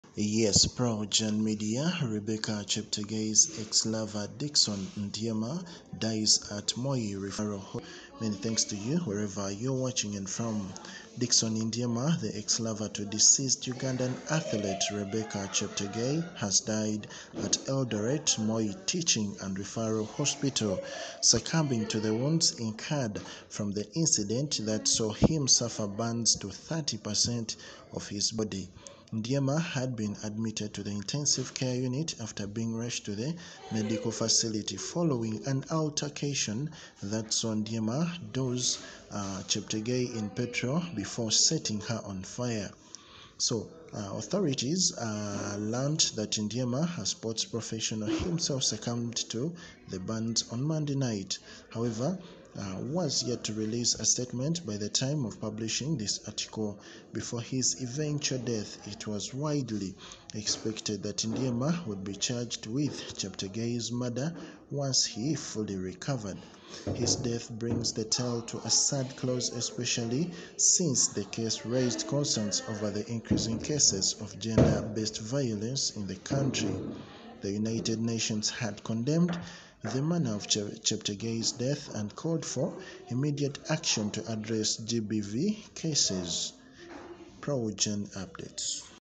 Rebecca Cheptegei's Ex-Lover Dickson Ndiema Dies at Moi Referral Hospital
Dickson Ndiema, the ex-lover to deceased Ugandan athlete Rebeca Cheptegei has died at Eldoret, Moi Teaching and Referral Hospital (MTRH), succumbing to the wounds incurred from the incident that saw him suffer burns to 30 per cent of his body.

Ndiema had been admitted to the Intensive Care Unit (ICU) after being rushed to the medical facility following an altercation that saw Ndiema douse Cheptegei in petrol before setting her on fire.Before his eventual death, it was widely expected that Ndiema would be charged with Cheptegei's murder once he fully recovered.

Kenyans.co.ke has authoritatively learnt that Ndiema, a sports professional himself succumbed to the burns on Monday night. MTRH, however was yet to release a statement by the time of publishing this article.